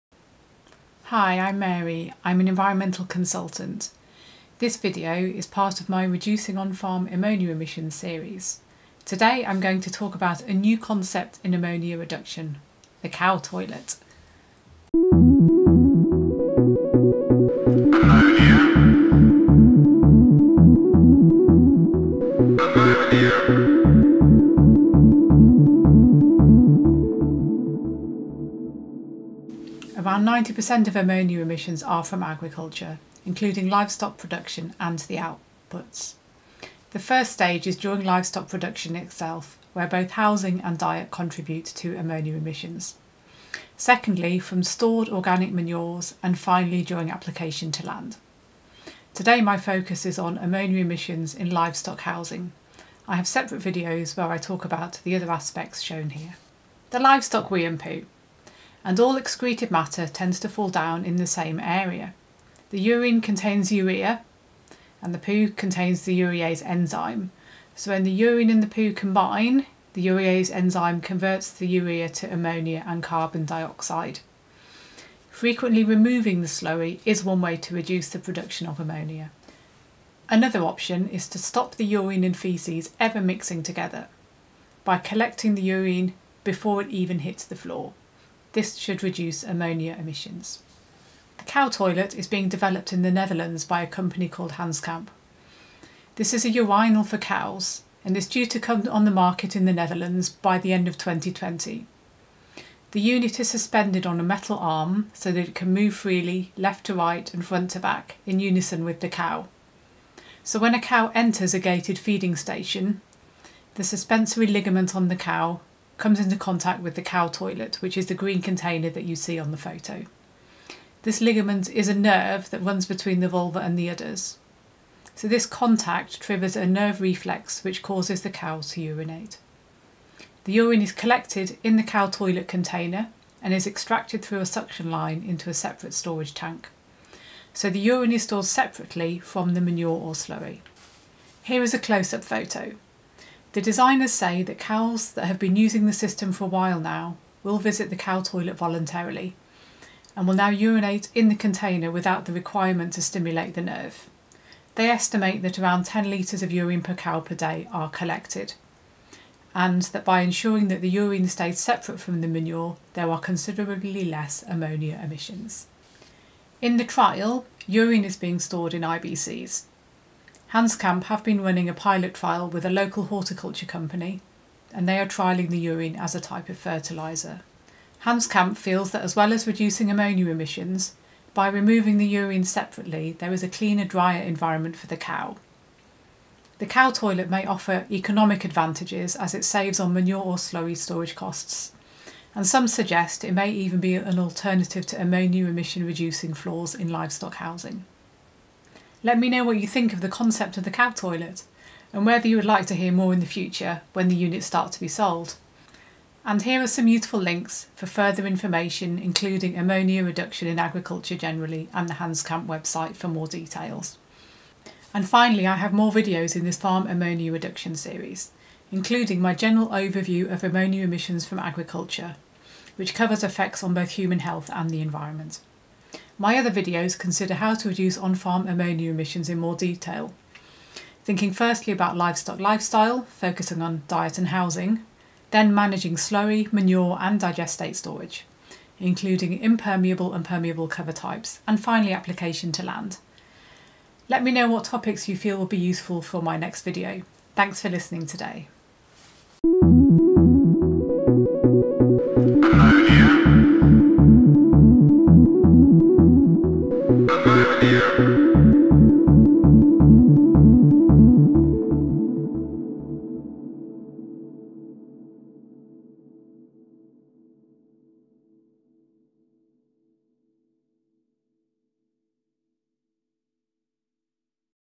H02 The cow toilet: Reducing on-farm ammonia emissions from livestock housing
This video is part of Mary’s ammonia emissions from agriculture series. This video provides an overview of how ammonia can be emitted from housed livestock. Mary then describes how the novel cow toilet concept (developed by Hanskamp) works, to collect urine separately and how could this reduce ammonia emissions and create a better environment for the housed livestock.
Let me know if you think this is a good idea for dairy cattle!